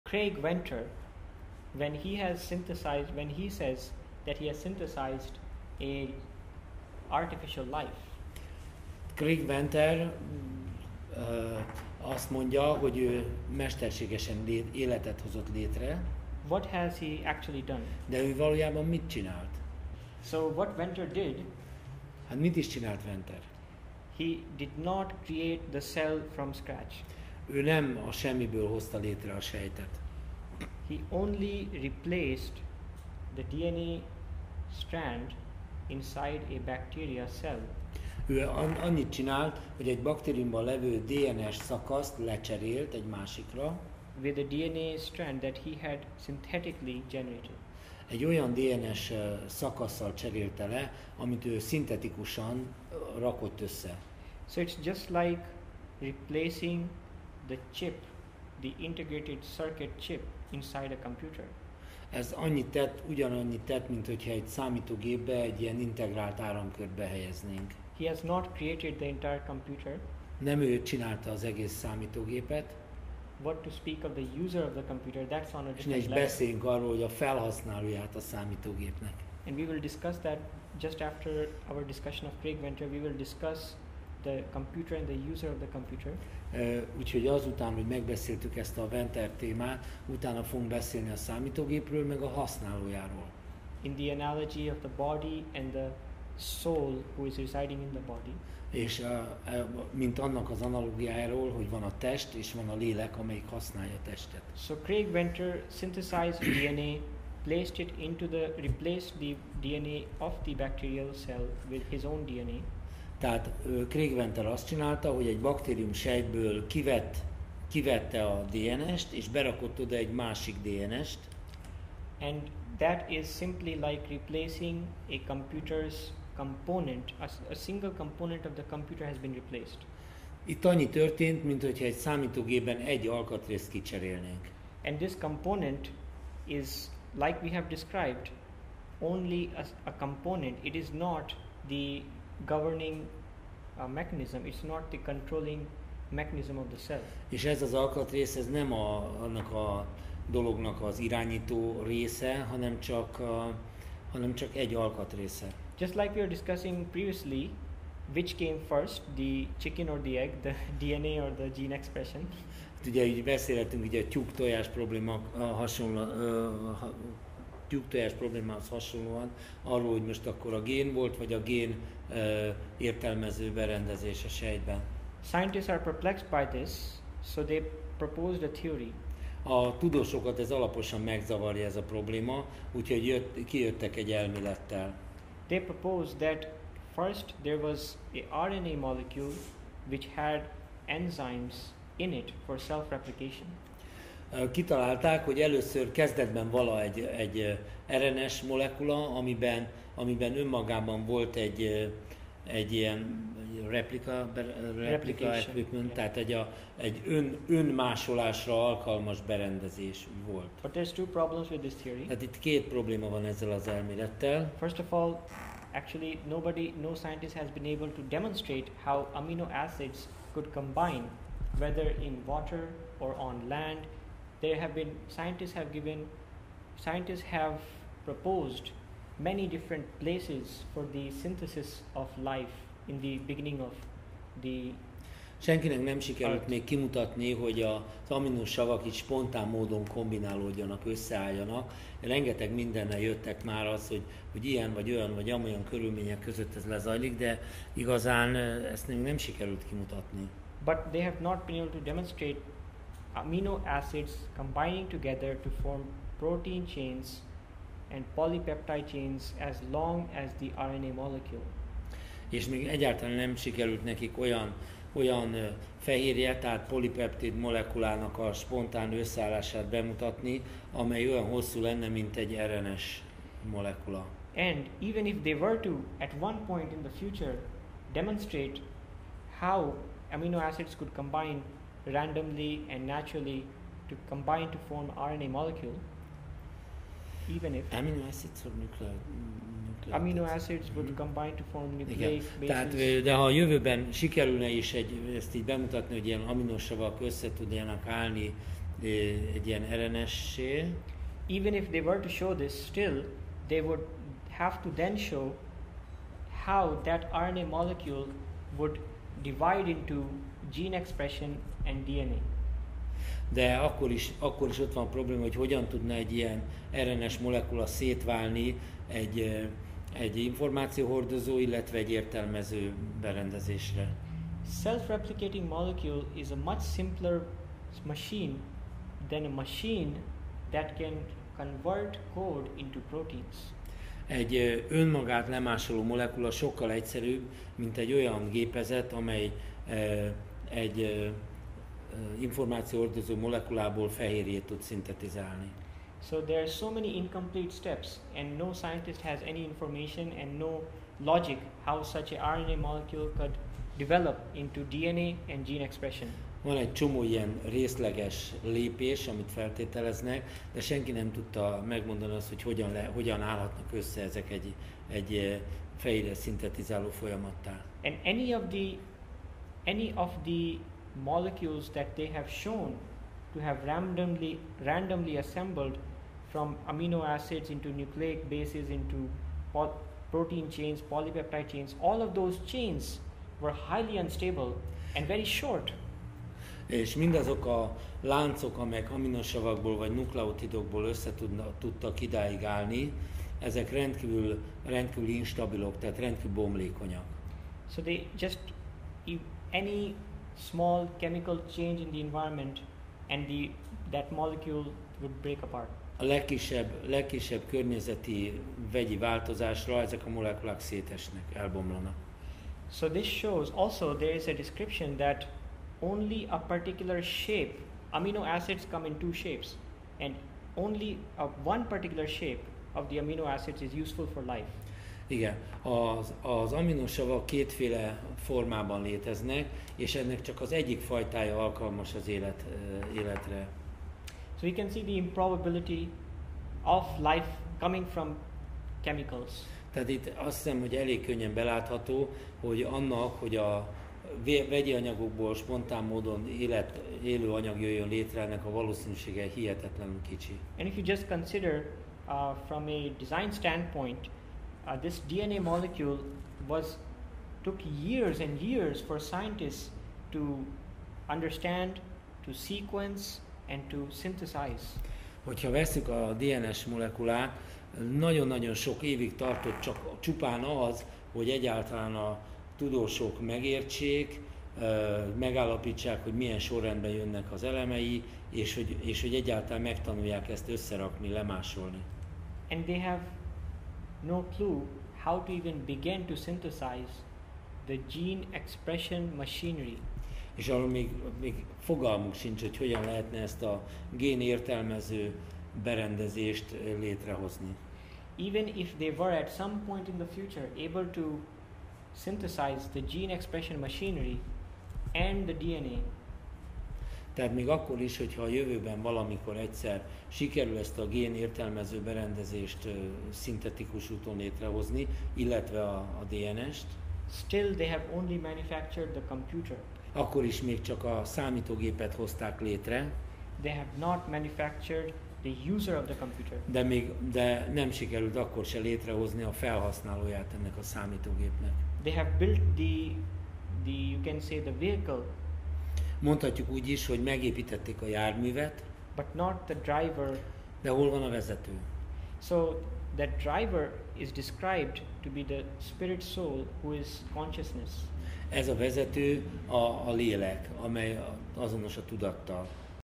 Craig Venter és a "mesterséges élet"
A hírneves molekuláris biológusnak, aki annak idején az emberi genom első térképét készítette, sikerült egy sejt magjába másolt, tehát mesterségesen előállított DNS szakaszt bevinnie, ami be is kapcsolódott a sejt fehérje szintézisébe.
A világsajtó szétkürtölte, hogy a tudósnak "mesterséges életet" sikerült létrehoznia. Valóban?